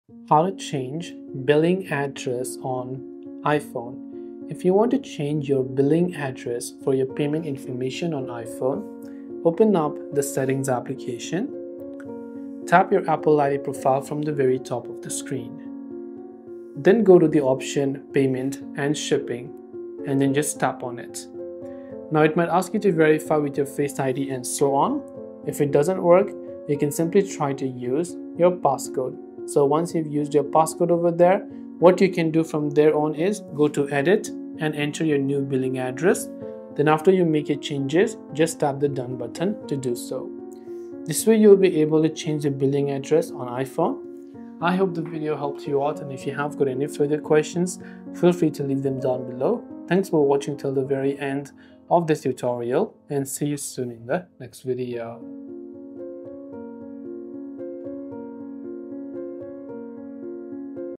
How to Change Billing Address on iPhone
Changing your billing address on your iPhone is an important step if you've recently moved, updated your payment method, or need to correct existing information for App Store or Apple Pay purchases. Apple allows you to easily update your billing details directly through the Settings app.

To begin, open the Settings app and tap on your Apple ID profile at the top of the screen. From the list of options, select Payment & Shipping. You may be asked to verify your identity using Face ID, Touch ID, or your device passcode, depending on your security settings.

Once inside the Payment & Shipping section, you’ll see your existing payment methods along with the associated billing address. Tap Edit next to the payment method you want to update. Here, you can modify the billing address, including the street, city, zip code, and country.

After entering the new billing address, tap Done to save your changes. These updates will now apply to purchases you make through the App Store, iCloud, and Apple services, and also sync across your Apple devices if you’re using the same Apple ID.

This process is essential for ensuring successful transactions, especially when using credit or debit cards that are sensitive to address mismatches during payment verification.

0:08 Accessing Apple ID settings
0:20 Opening Payment & Shipping section
0:32 Editing and saving your billing address
0:42 Conclusion